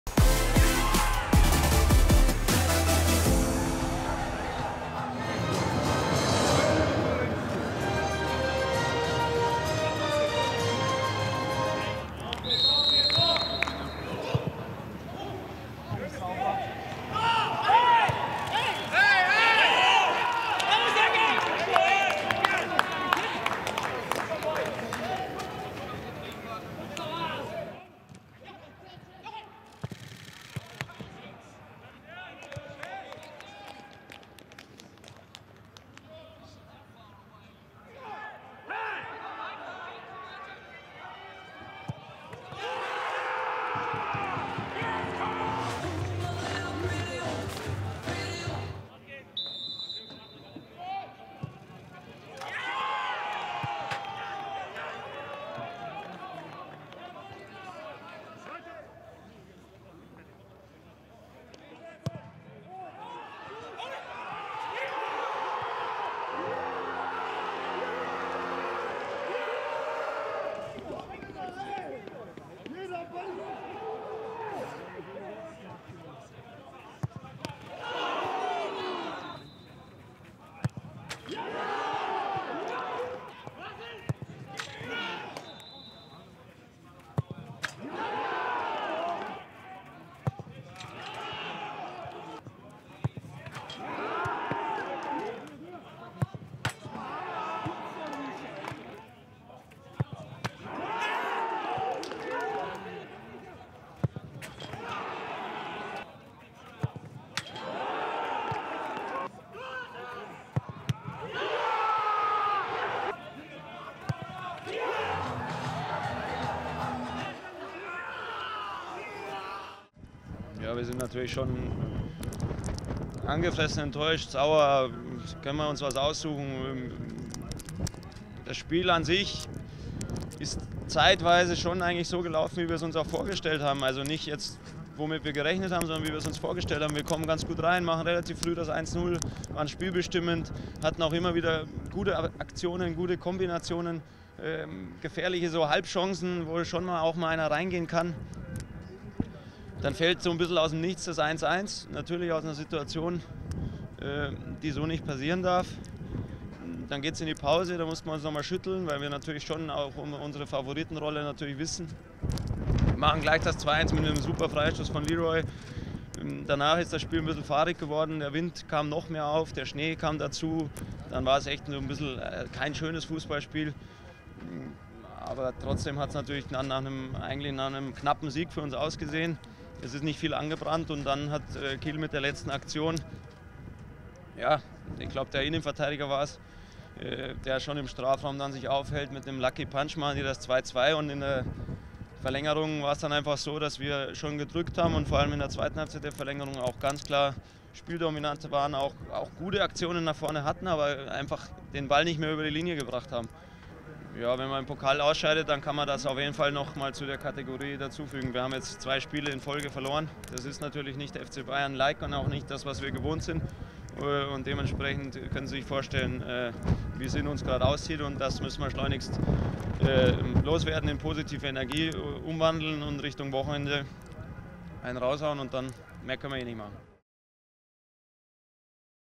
Nice Free Kick Goal by Leroy Sané | Highlights Holstein Kiel vs. FC Bayern 8-7 Pens | DFB-Pokal
FC Bayern Munich lose to Holstein Kiel after penalties in the 2nd Round of the DFB-Pokal. Check out the highlights of the match including the goals by Serge Gnabry and Leroy Sané as well as the post-game interview by Thomas Müller.